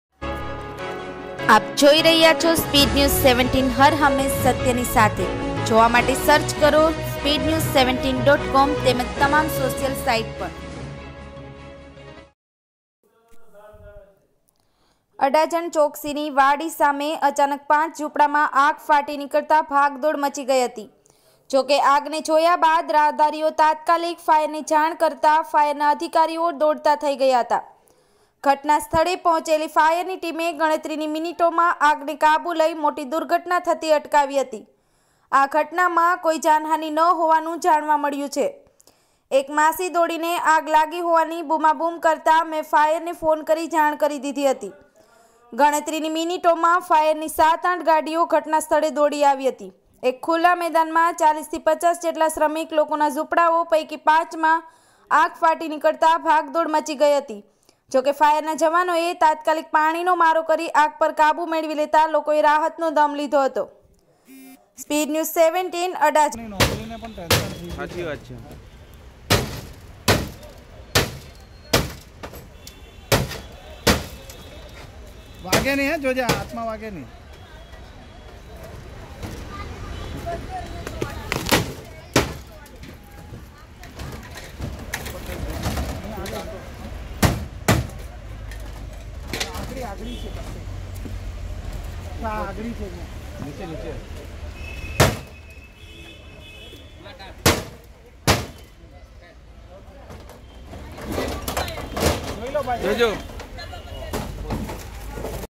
SPEEDNEWS17 અડાજણ ચોકસીની વાડી સામે અચાનક 5 ઝૂંપડામાં આગ ફાટી નીકળતા ભાગદોડ મચી ગઇ હતી.
અડાજણ ચોકસીની વાડી સામે અચાનક 5 ઝૂંપડામાં આગ ફાટી નીકળતા ભાગદોડ મચી ગઇ હતી. જોકે આગ ને જોયા બાદ રાહદારી એ તાત્કાલિક ફાયર ને જાણ કરતા ફાયર ના અધિકારીઓ દોડતા થઈ ગયા હતા. ઘટના સ્થળે પહોંચેલી ફાયરની ટીમે ગણતરીની મિનિટોમાં આગ ને કાબૂમા લઈ મોટી દુર્ઘટના થતી અટકાવી હતી. આ ઘટનામાં કોઈ જાનહાની ન હોવાનું જાણવા મળ્યું છે.

  એક માસી દોડી ને આગ લાગી હોવાની બુમાબુમ કરતા મેં ફાયર ને ફોન કરી જાણ કરી દીધી હતી. ગણતરીની મિનિટોમાં ફાયર ની 7-8 ગાડીઓ ઘટના સ્થળે દોડી આવી હતી. એક ખુલ્લા મેદાનમાં 40-50 જેટલા શ્રમિક લોકોના ઝૂંપડાઓ પૈકી 5 મા આગ ફાટી નીકળતા ભાગદોડ મચી ગઇ હતી. જોકે ફાયરના જવાનોએ તાત્કાલિક પાણી નો મારો કરી આગ પર કાબુ મેળવી લેતા લોકોએ રાહતનો દમ લીધો હતો.